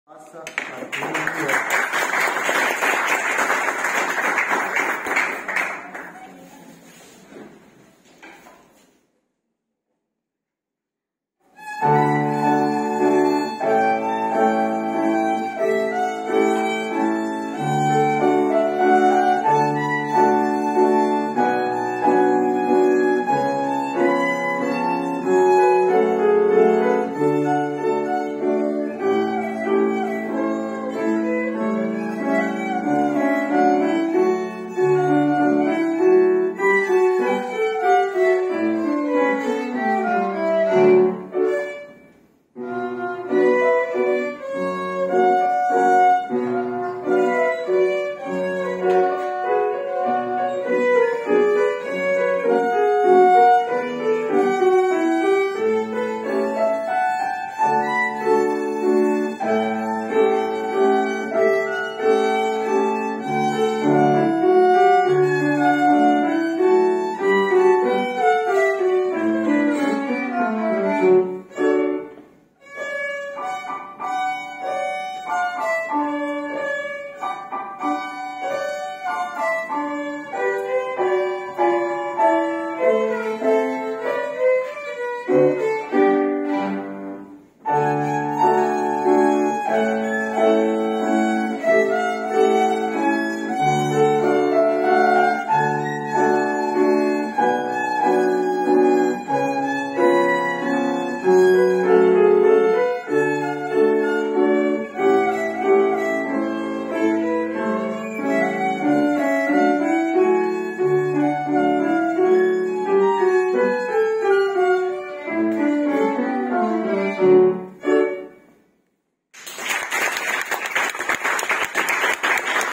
Ученик на интерном часу 17
У Музичкој школи Корнелије Станковић је 27. децембра 2023. године са почетком у 18 часова одржан јавни интерни час за ученике ове школе.